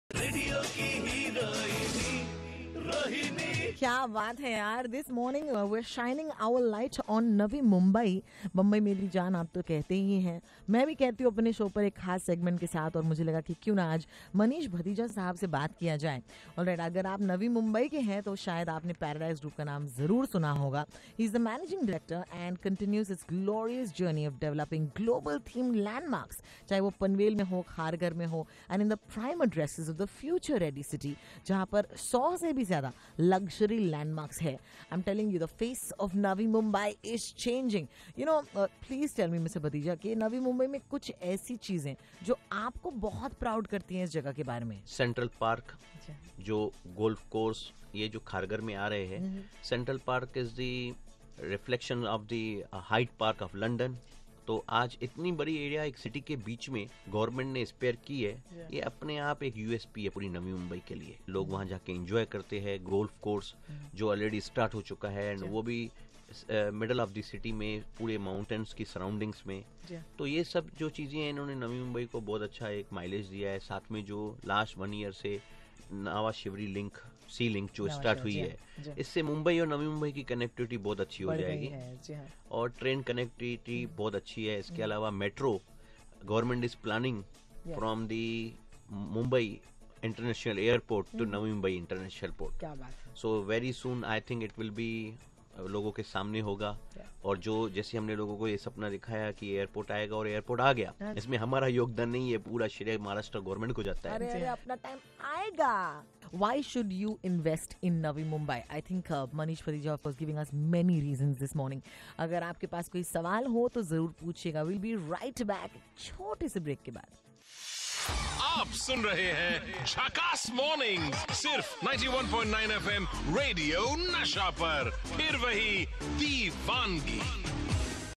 Interview with Radio Nasha - Part 3
Check out the interview of our Managing Director, Shri. Manish Bathija with Radio Nasha